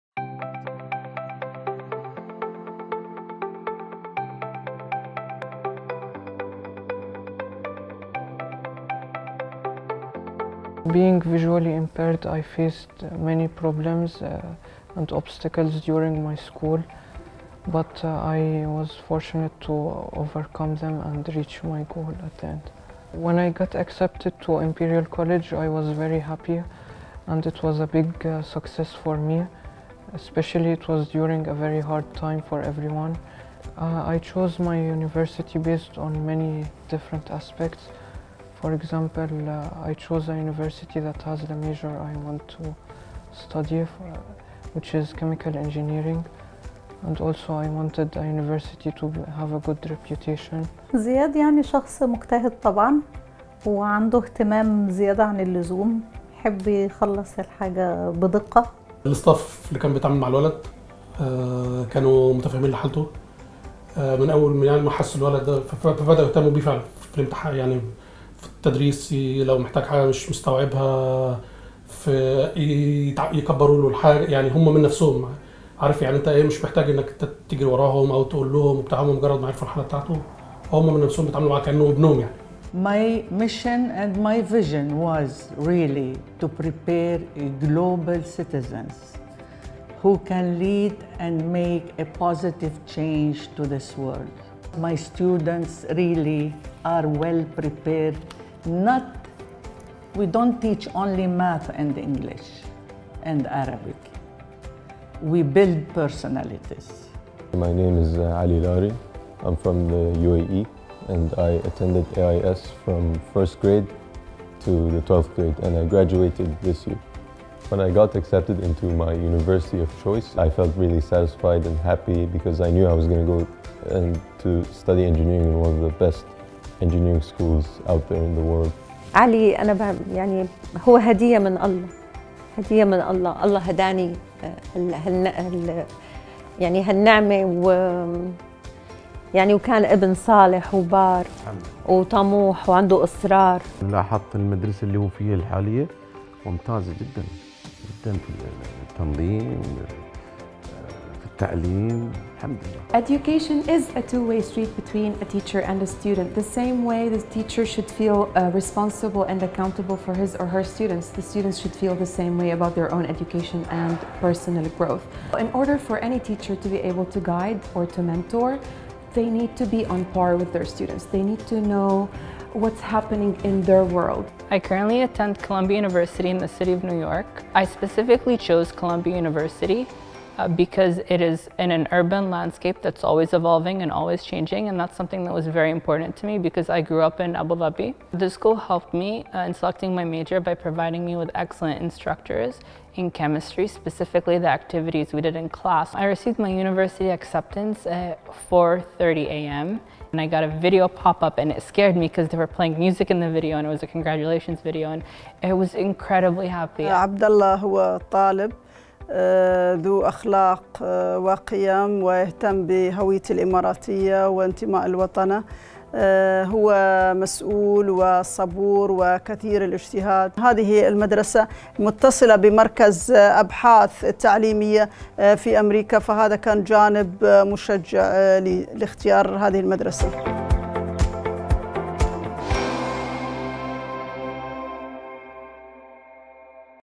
Student Success Stories - Abu Dhabi International Private School
تعرفوا على قصص نجاح ثلاثة من خيرة طلبة أبوظبي

تم قبول الطالب المتميز زياد إبراهيم، وعلي لاري وعبدالله بن تميم للدراسة في ثلاث جامعات عالمية رائدة، بفضل تفوقهم الأكاديمي واجتهادهم، ونتيجة للدعم الذي تلقوه من مدرسة أبوظبي الدولية الخاصة مدرسة أبوظبي الدولية الخاصة

Meet three exceptional and inspiring Abu Dhabi students sharing their educational success stories -

Zeyad Ibrahim, a brilliant student of determination, Ali Lari and Abdullah Bin Tamim were accepted to study at three of the world’s leading universities, thanks to their academic excellence, hard work and support from Abu Dhabi International Private School